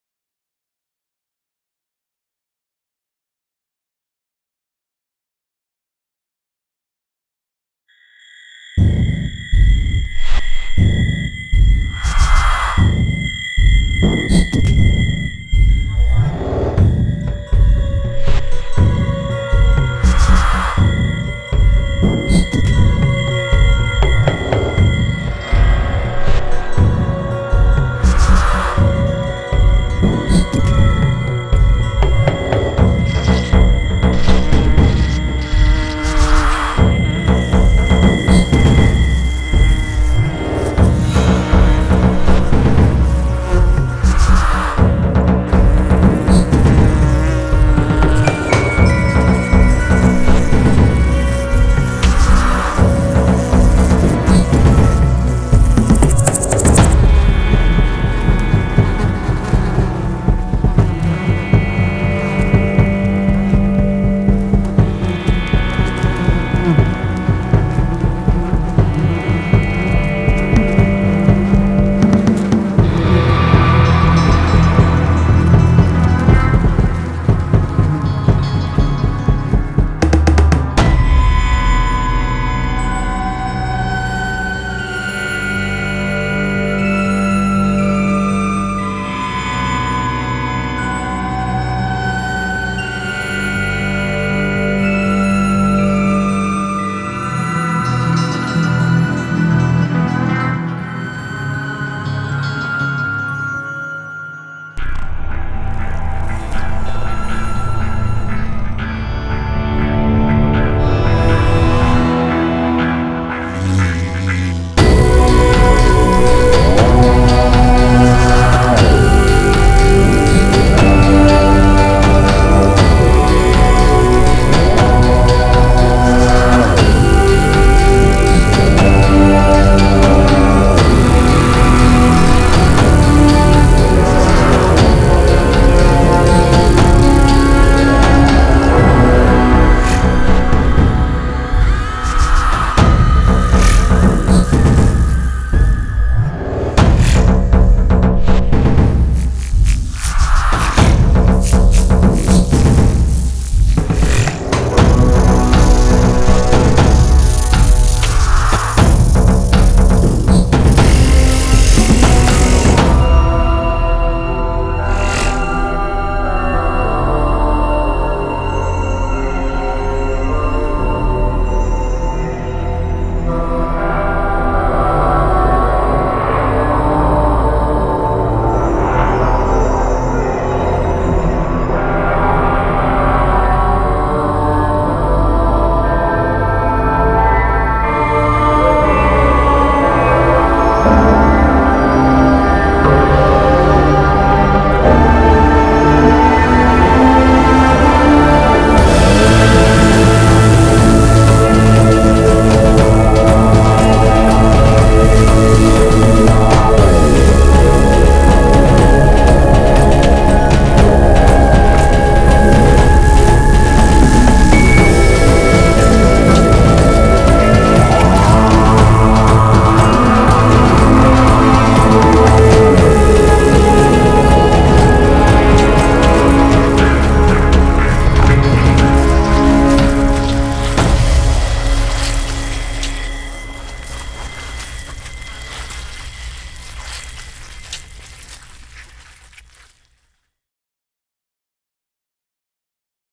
OFFF-Trailer 2010 - Musical adaption by Simon Sanchez
I was so impressed by this visual masterpiece, I had to write my own music & sound-design for it. Here is my remix or work specimen for the OFFF-Trailer 2010.